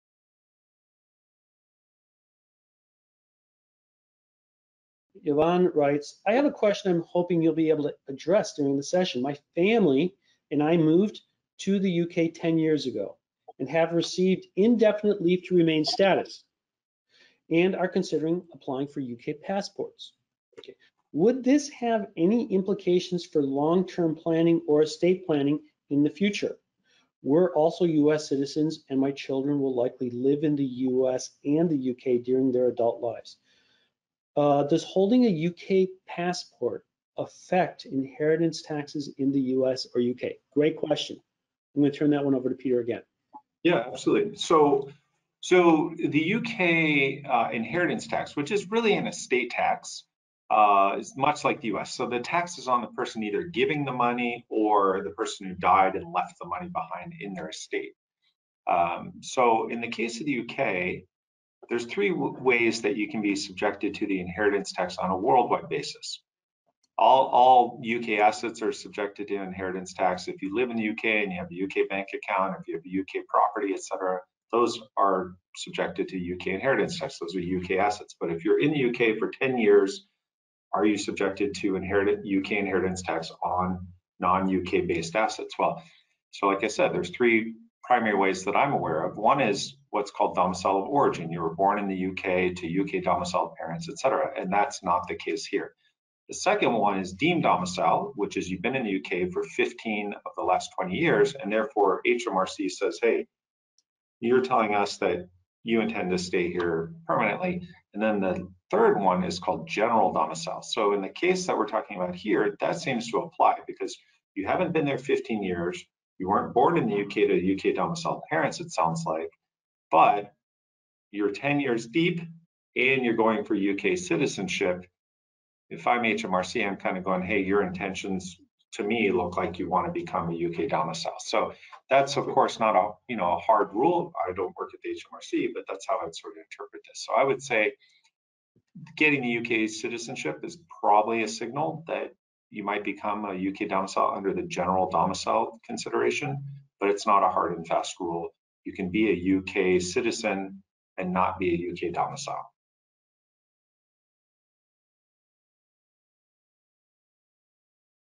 Does Holding a UK Passport Affect Inheritance Taxes in the U.S. or UK? | Ask an Intl Wealth Manager
Does becoming a UK citizen affect inheritance taxes in either the U.S. or the UK? In this video, we explain the three ways UK domicile may be determined--and how much of your UK assets would be subject to UK inheritance tax as a U.S. expat.